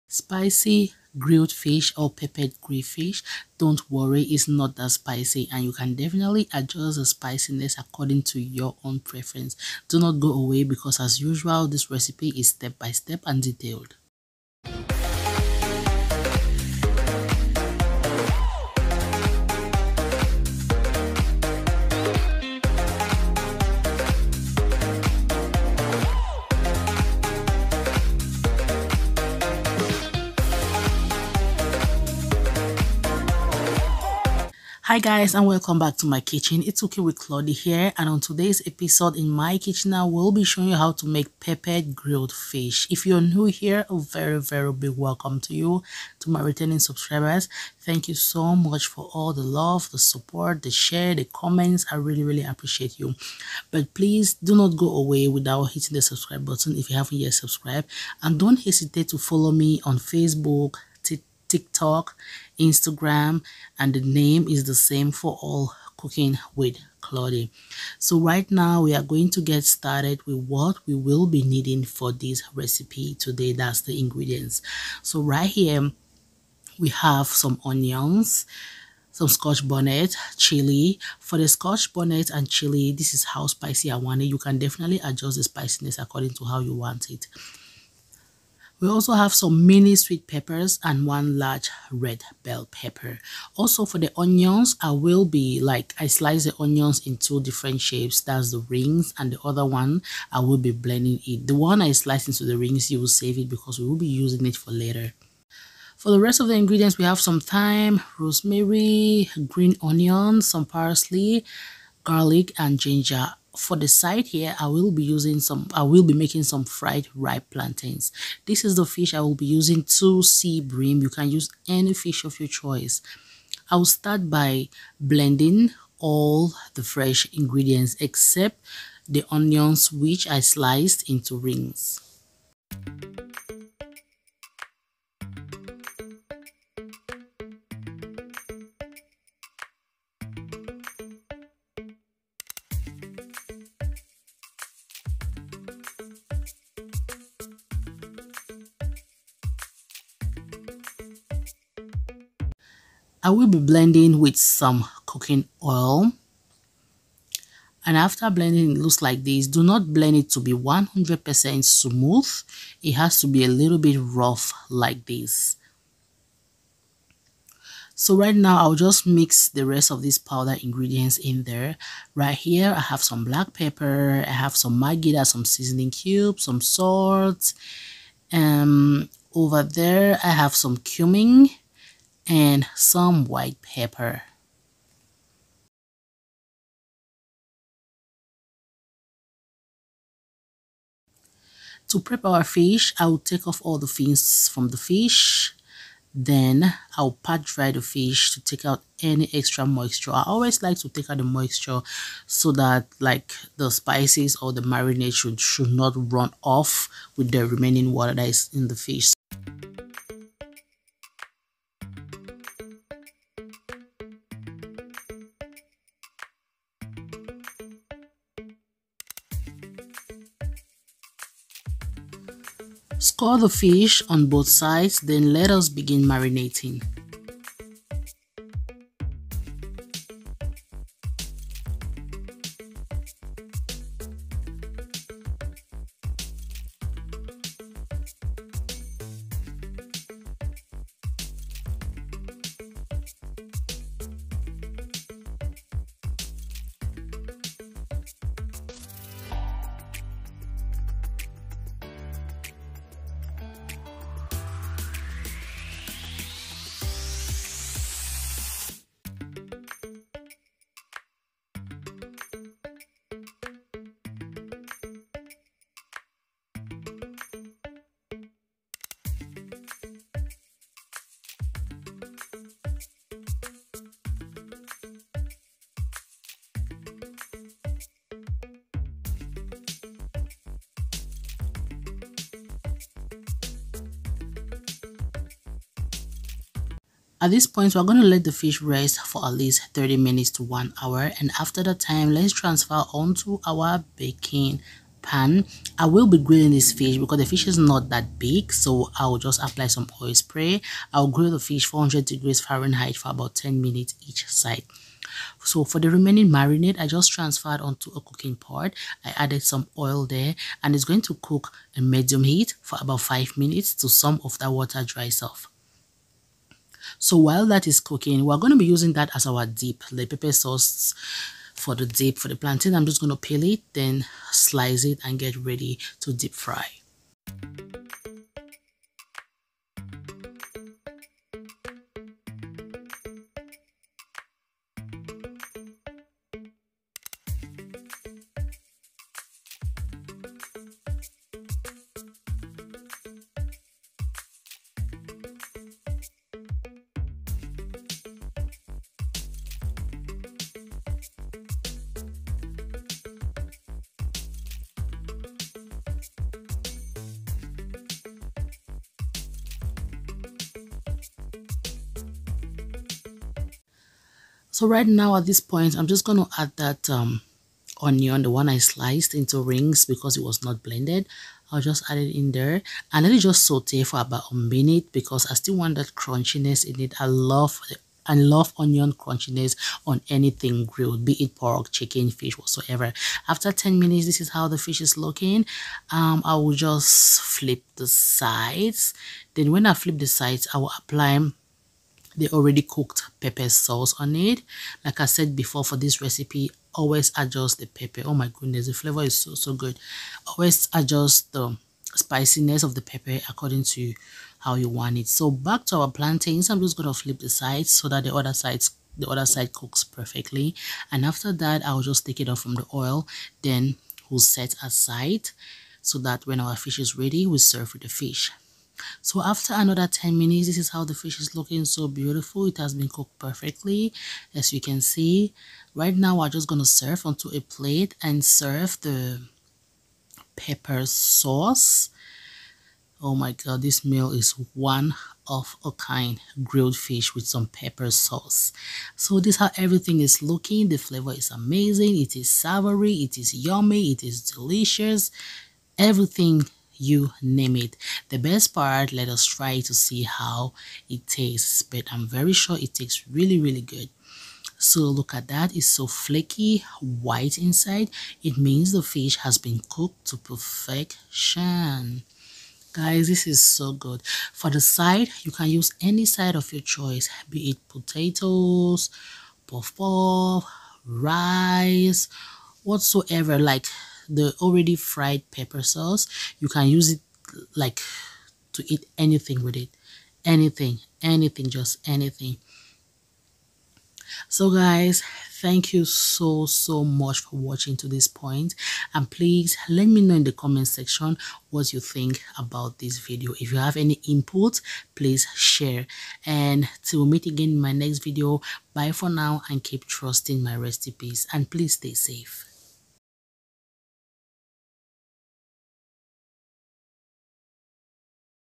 PEPPERED GRILLED FISH RECIPE
2 Medium Size Sea Bream. Scaled and Gutted
1 Onion ( Slice half and Blend half)
1 Large Red Bell Pepper
2 Mini Sweet Pepper
2 Scotch Bonnets
4 Chilli Pepper
3 Sprigs of Thyme
1 Rosemary Sprig
1 Green Onion
4 Garlic Cloves
½ Thumpsize Ginger
4 Stems Parsley or Cilantro
1 tsp Salt
1 tsp Blackpepper
1 tsp Whitepepper
1 tsp Cumin Powder
2 tsp Bouillon Powder
3 Ripe Plantains